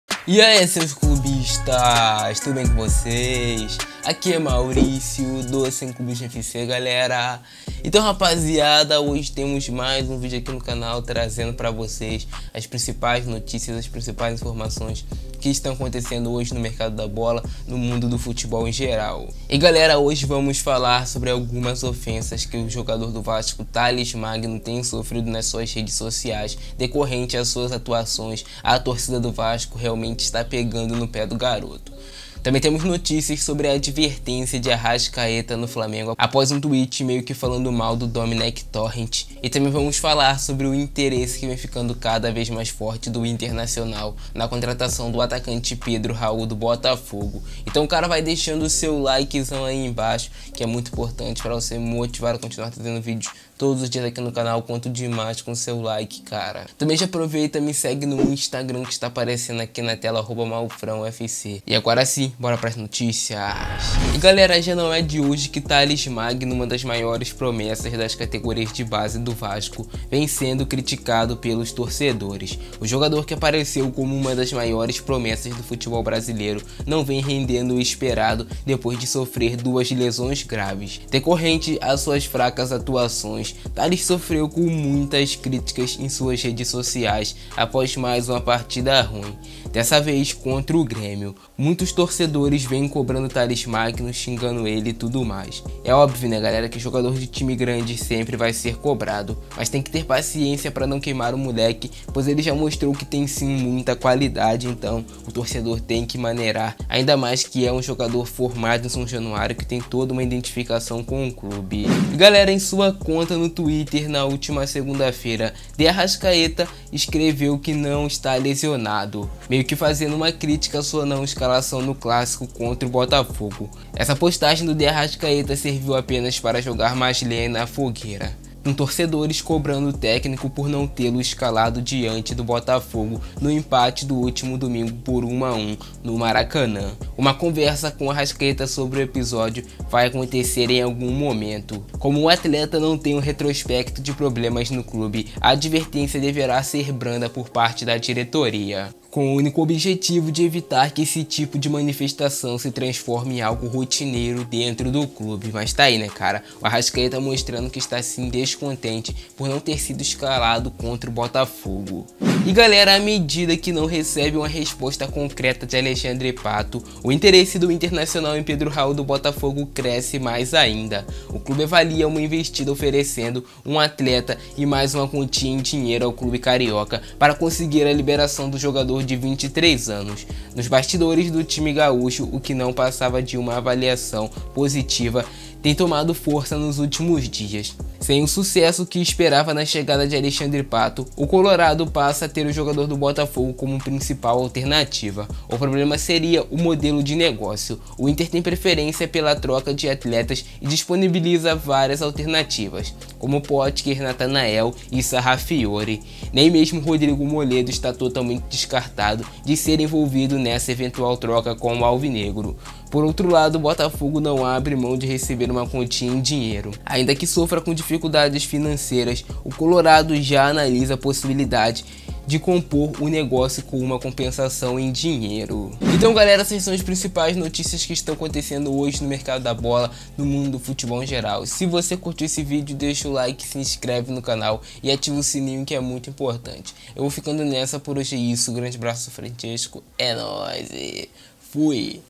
TALLES MAGNO É ATACADO POR VASCAINOS NAS REDES SOCIAIS/ INTER VAI TIRAR PEDRO RAUL DO BOTAFOGO?
Então galera hoje vamos falar sobre os ataque que talles magno vem sofrendo em suas redes sociais, o jogador do vasco talles magno vem sendo atacado por torcedores vascainos decorrente as suas fracas atuações, também vamos fala sobre a advertencia que vai sofrer arrascaeta no flamengo, e vamos falar sobre o interesse forte do internacional na contratação de pedro raul do botafogo, sera que pedro raul realmente vai pro internacional?

LOJA